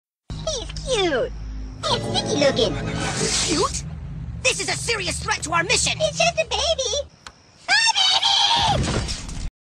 MMD - HI BABY!
Man, I am BUSTING OUT VIDEOS TODAY!  Someone even pointed that out to me. XD  This meme is one that I really love!  I think Shadow really fits Zim in these Invader Zim memes.